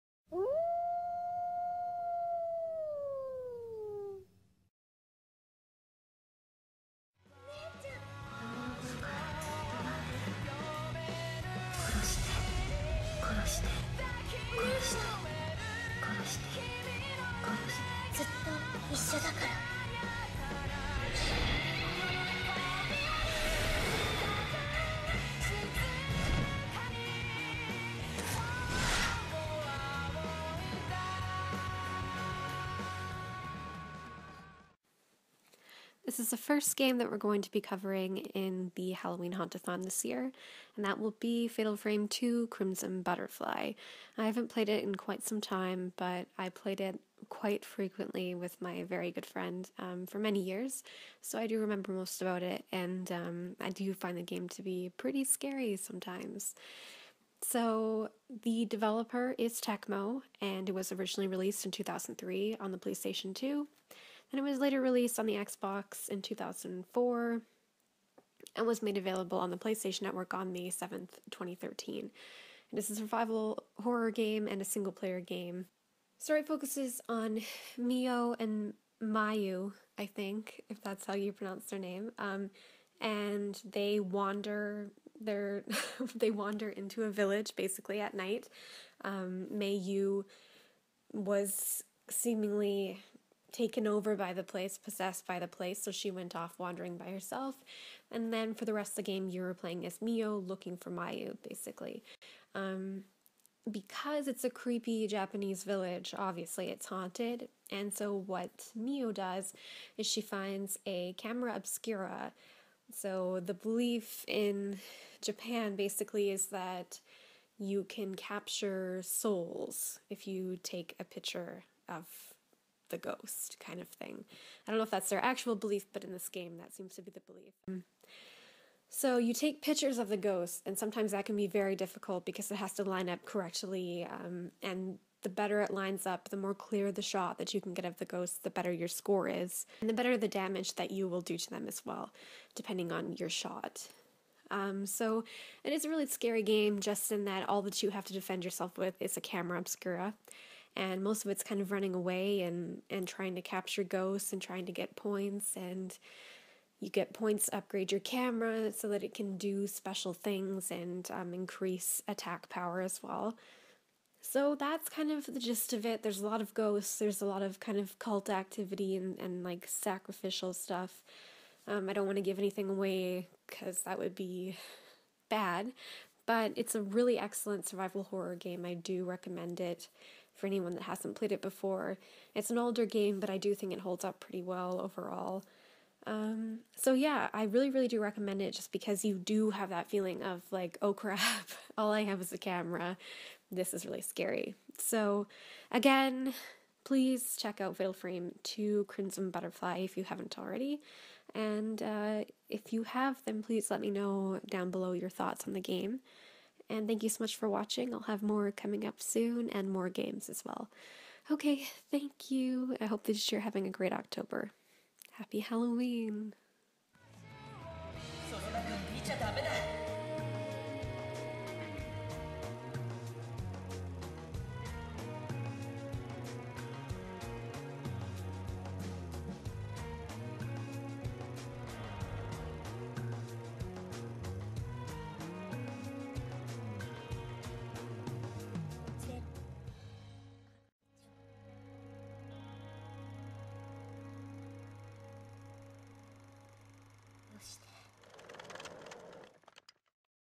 Halloween Hauntathon: Fatal Frame II: Crimson Butterfly
Review of Fatal Frame II: Crimson Butterfly which was released by Tecmo in 2003. The opinion, however is mine. I appreciate all your support.

Fatal Frame Review
Fatal Frame
Halloween Marathon
Best games for Halloween
Halloween Hauntathon
Halloween